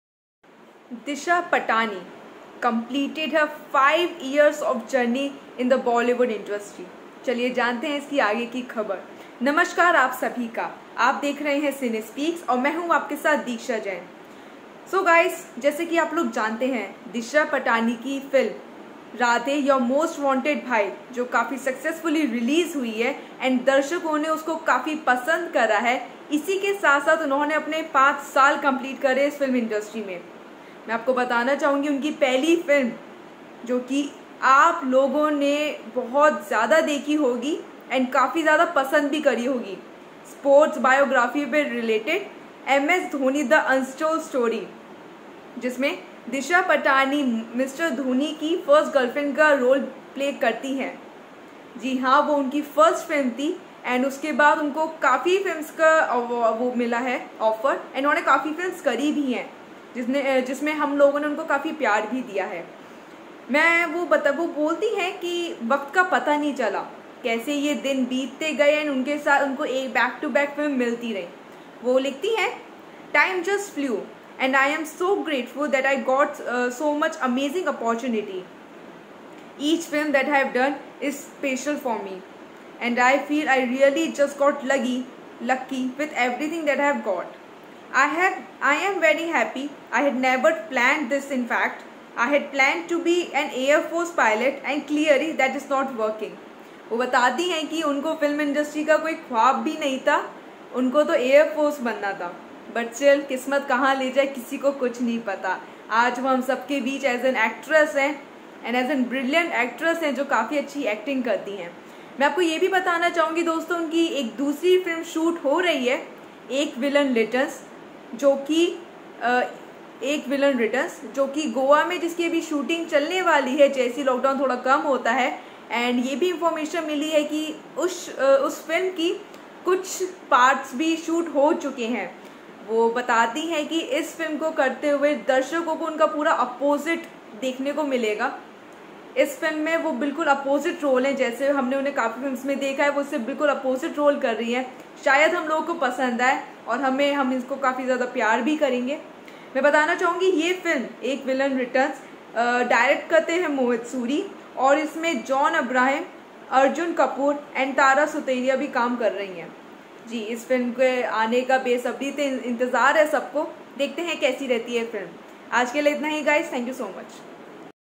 Kis film mei dikhnr wali h Disha Patani
Kesai role mein dikhengyi Disha Patani, jantae h iss video mei.